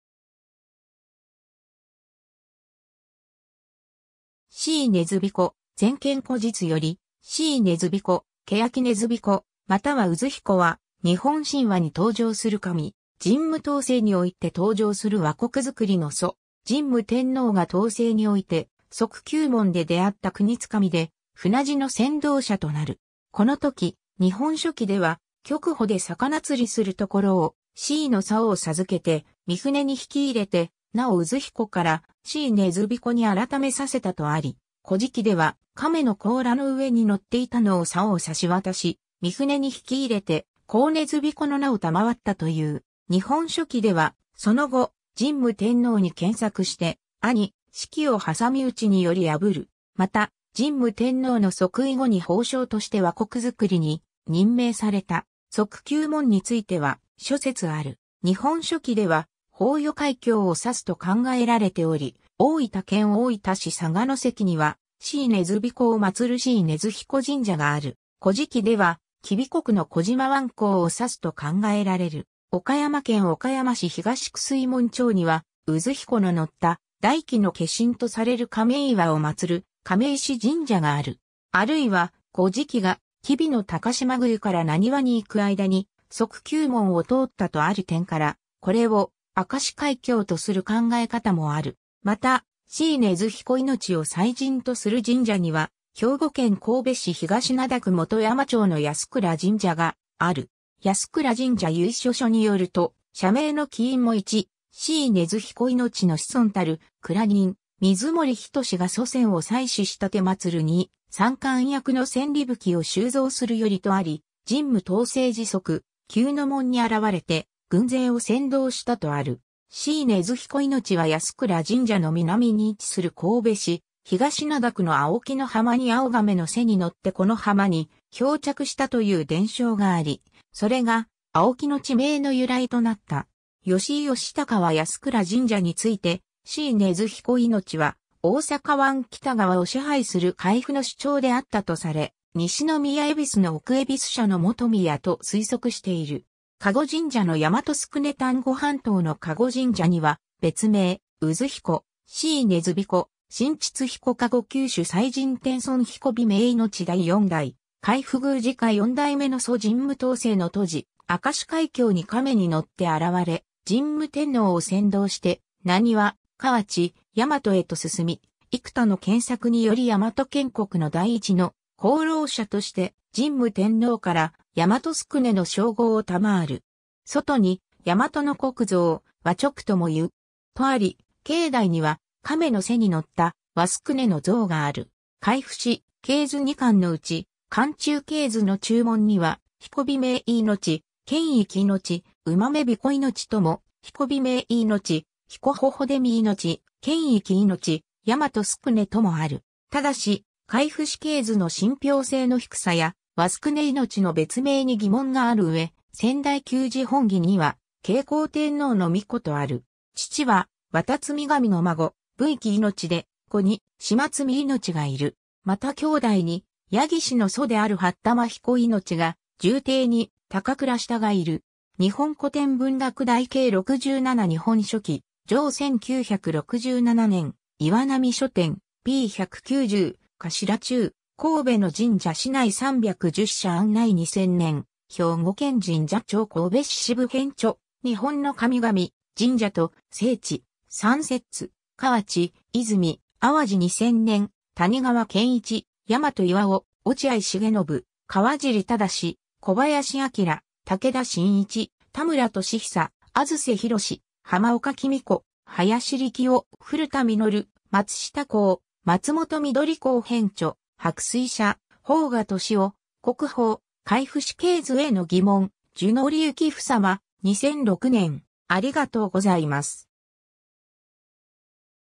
椎根津彦
日本の神_(人物神_記紀)
椎根津彦　／『前賢故実』より 椎根津彦（しいねつひこ、『日本書紀』）、槁根津日子（さおねつひこ、『古事記』）、または珍彦（うずひこ）は、日本神話に登場する神。
神武東征において登場する倭国造（倭直部）の祖。
 神武天皇が東征において速吸門で出会った国津神で、船路の先導者となる。
このとき、『日本書紀』では曲浦（わだのうら）で魚釣するところを椎の棹を授けて御船に引き入れて名を珍彦（うづひこ）から椎根津彦に改めさせたとあり、『古事記』では亀の甲羅の上に乗っていたのを棹をさし渡し御船に引き入れて槁根津日子の名を賜ったという。
 『日本書紀』ではその後、神武天皇に献策して兄磯城を挟み撃ちにより破る。
また神武天皇の即位後に褒賞として倭国造に任命された。
 速吸門については諸説ある。
『日本書紀』では豊予海峡を指すと考えられており、大分県大分市佐賀関には、椎根津彦を祀る椎根津彦神社がある。
『古事記』では吉備国の児島湾口を指すと考えられる。
岡山県岡山市東区水門町には、珍彦（宇豆毘古命、うづひこのみこと）の乗った大亀の化身とされる亀岩を祀る亀石神社（かめいわじんじゃ）がある。
あるいは『古事記』が吉備の高島宮から浪速に行く間に速吸門を通ったとある点から、これを明石海峡とする考え方もある。
 また、椎根津彦命を祭神とする神社には、兵庫県神戸市東灘区本山町の保久良神社がある。
保久良神社由緒書によると「社名の起因も 1、椎根津彦命の子孫たる倉人水守等が祖先を祭祀し奉る 2、三韓役の戦利武器を収蔵するより」とあり、神武東征時速吸之門（明石海峡）に現れて軍勢を先導したとある。
 椎根津彦命は保久良神社の南に位置する神戸市東灘区の青木（おうぎ）の浜に青亀（おうぎ）の背にのってこの浜に漂着したという伝承があり、それが青木（おうぎ）の地名の由来となった。
 吉井良隆は保久良神社について「椎根津彦命は大阪湾北側を支配する海部の首長であったとされ、西宮夷（兵庫県西宮市西宮神社）の奥夷社の元宮」と推測している。
 籠神社の倭宿禰 丹後半島の籠神社（このじんじゃ）には「別名・珍彦・椎根津彦・神知津彦 籠宮主祭神天孫彦火明命第四代　海部宮司家四代目の祖　神武東征の途次、明石海峡（速吸門）に亀に乗って現れ、神武天皇を先導して浪速、河内、大和へと進み、幾多の献策に依り大和建国の第一の功労者として、神武天皇から倭宿禰（やまとすくね）の称号を賜る。外に大倭国造、倭直とも云う。」とあり、境内には亀の背に乗った倭宿禰の像がある。
 海部氏系図二巻のうち『勘注系図』の註文には「彦火明命―建位起命―宇豆彦命（うずひこ）」とも「彦火明命―彦火火出見命―建位起命―倭宿祢」ともある。
ただし海部氏系図の信憑性の低さや倭宿禰命の別名に疑問がある上、『先代旧事本紀』には景行天皇の皇子とある。
 父は綿津見神の孫・武位起命で、子に志麻津見命がいる。
また兄弟に八木氏の祖である八玉彦命が、従弟に高倉下がいる。